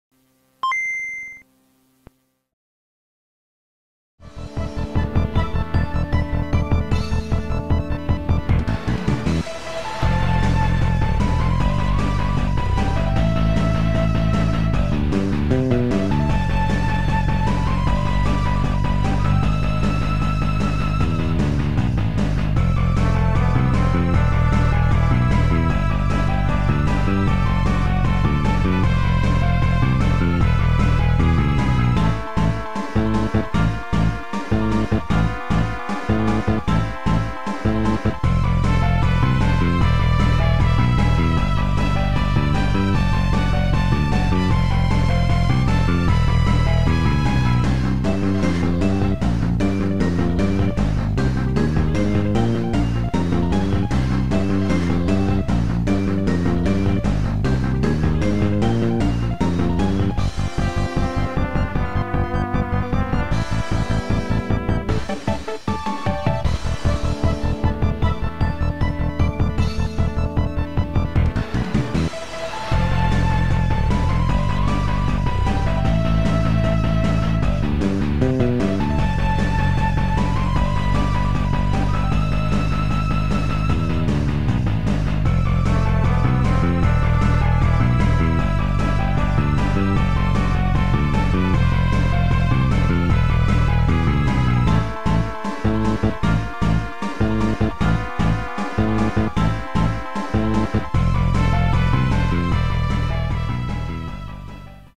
Ariel Levy vs Chris Farrow (GBA PROMO) - CCW World Heavyweight Championship
If Super Nintendo/Gameboy Advanced made games still lol

ARIEL LEVY vs CHRIS FARROW tonight in Homestead 🔥